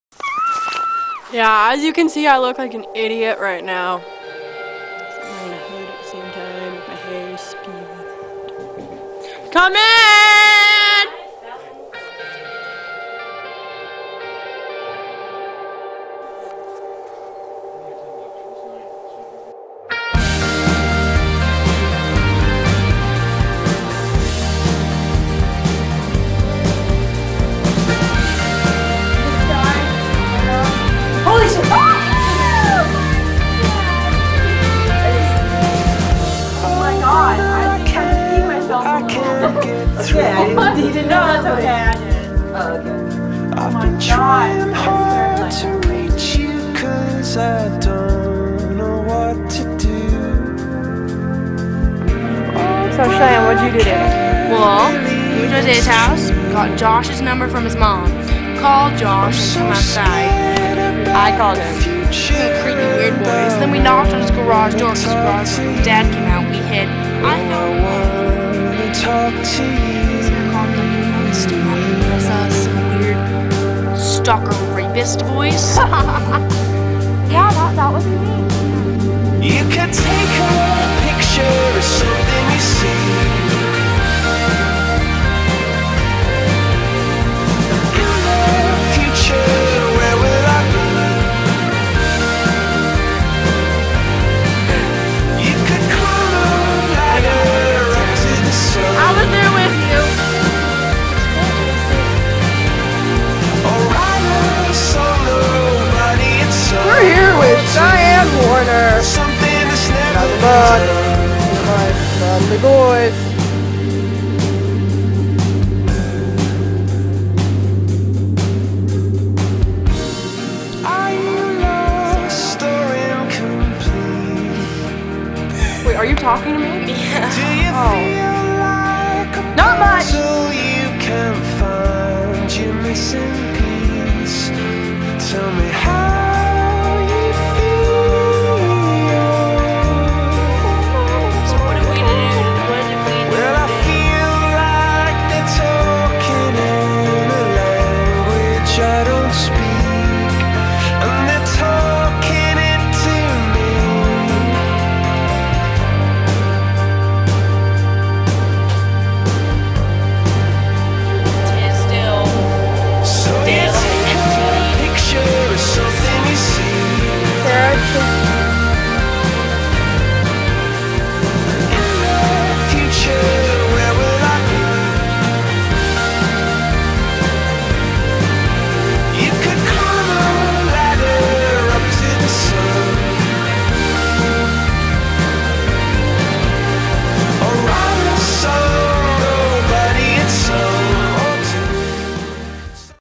Sarah Thompson and Freinds!!!
A whole bunch of cool pictures and funny video of cheyanne, misty , kaillani, and sarah!